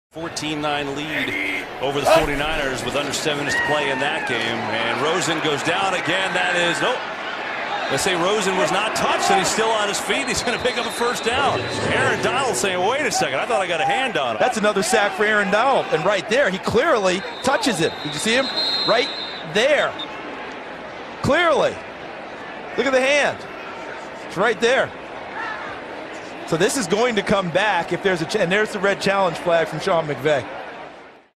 Aaron Donald sets Rams' franchise record with sack No. 19.5
Los Angeles Rams defensive tackle Aaron Donald picks up his third sack of the day, which allows him to surpass Robert Quinn for the franchise record in sacks for a single season.